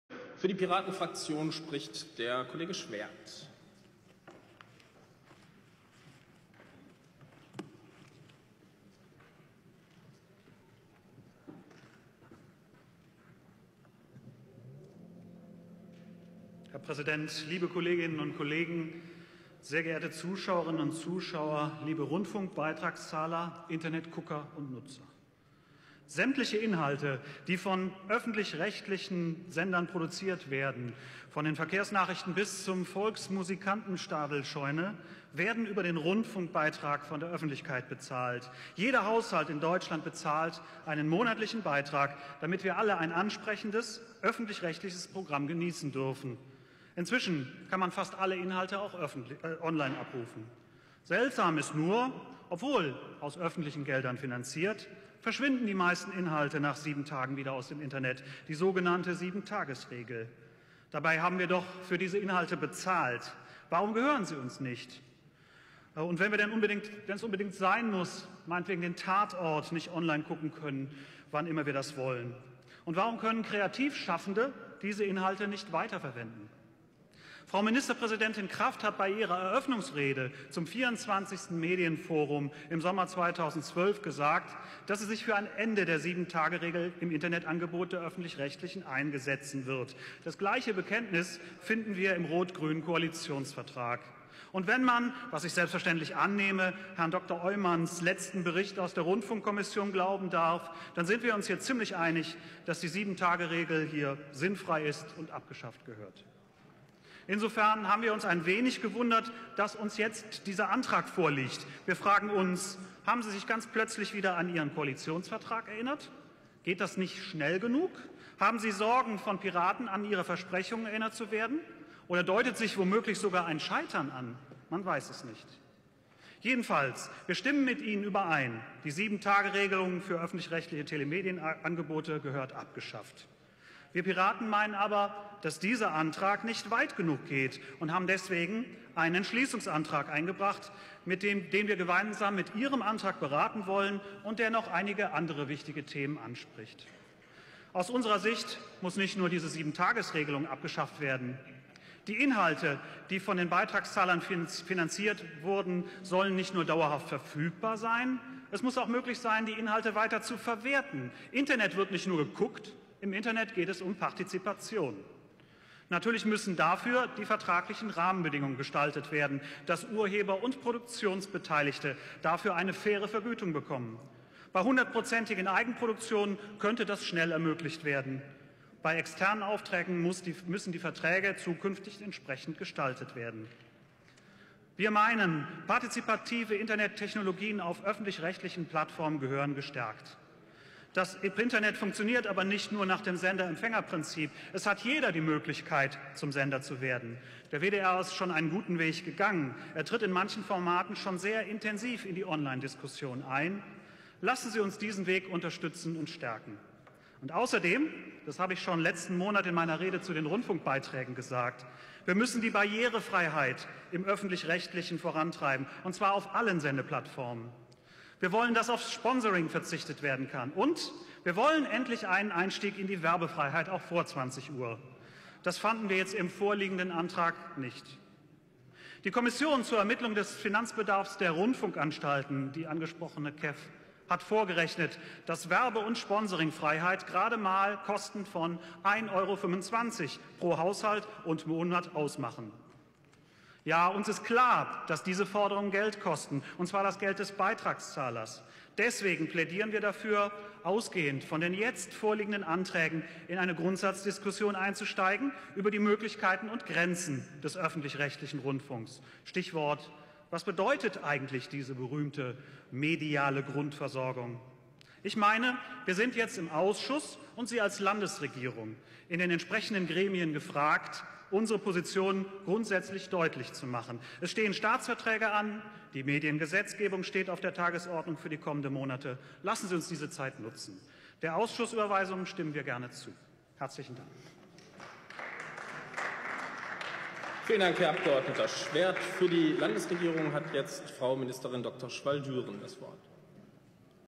Daniel Schwerd (@netnrd) über Telemedienangebot für Beitragszahlerzahler verbessern
Plenarsitzung 49 vom Donnerstag, 30. Januar 2014

Top 6. Öffentlich-rechtliches Telemedienangebot für Beitragszahlerinnen und -zahler verbessern (Abschaffung der 7-Tage-Frist)

Drucksache 16/4809